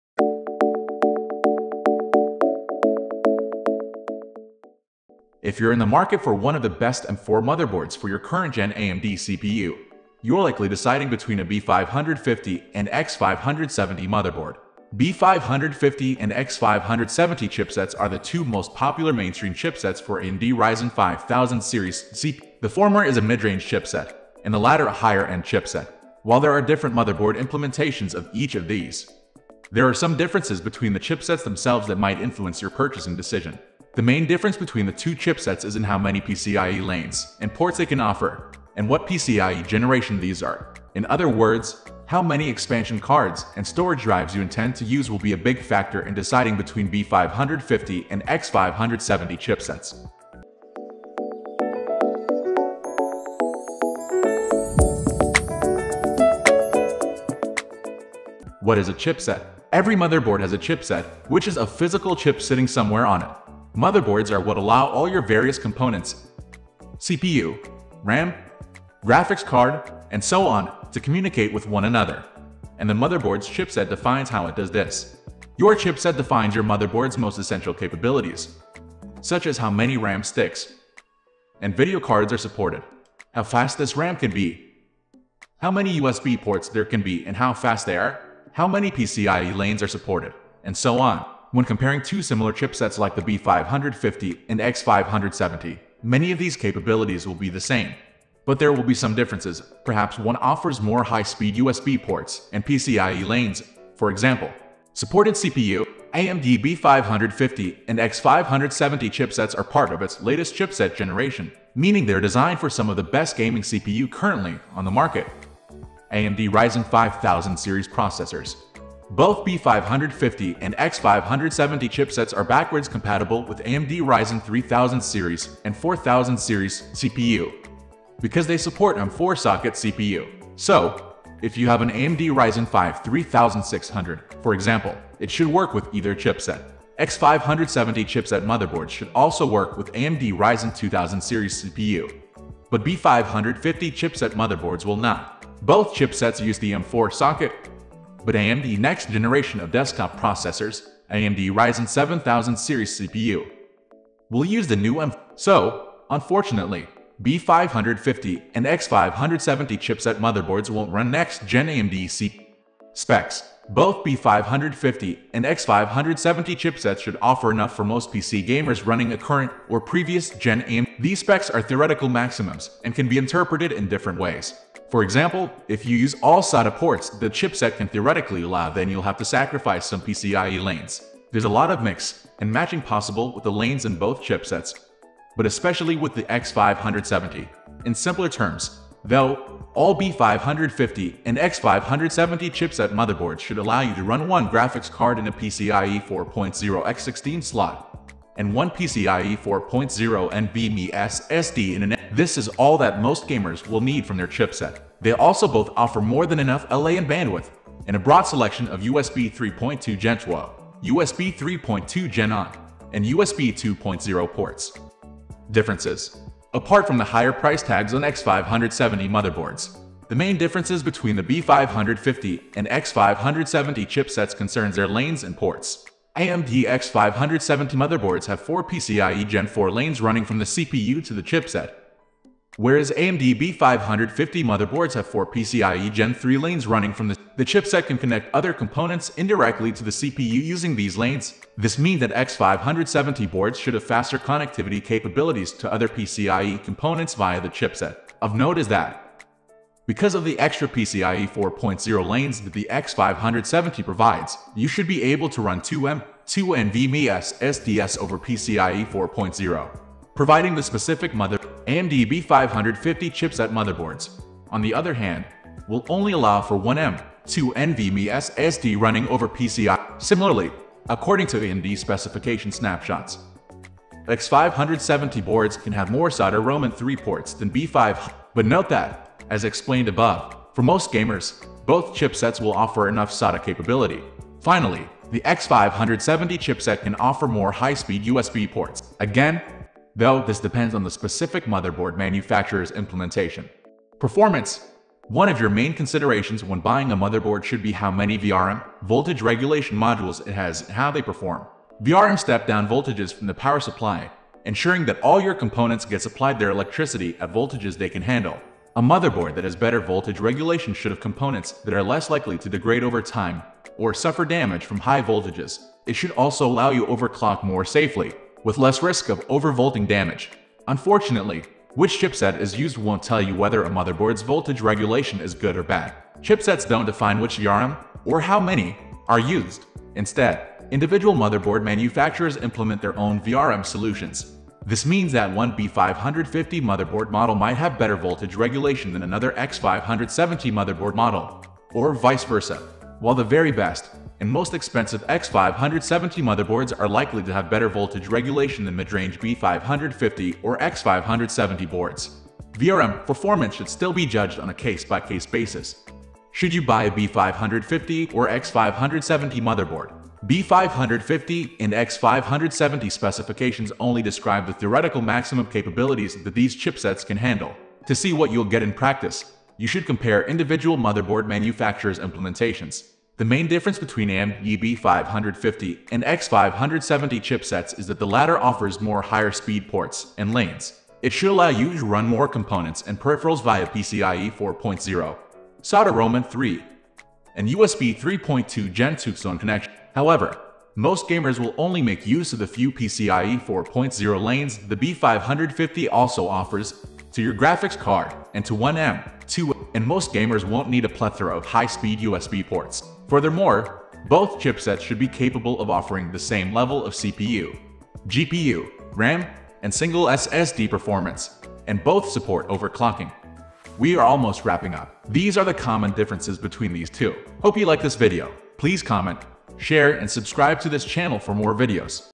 AMD B550 Vs X570 Motherboard: Which Chipset is Best? | Which to Buy?
The main difference between the two chipsets is in how many PCIe lanes and ports they can offer, and what PCIe generation these are. In other words, how many expansion cards and storage drives you intend to use will be a big factor in deciding between B550 and X570 chipsets.

B550:
1 . MSI MPG B550 GAMING PLUS Gaming Motherboard

X570:
1 . GIGABYTE X570 UD ATX Motherboard
2. ASUS ROG STRIX B550-F Gaming ATX Motherboard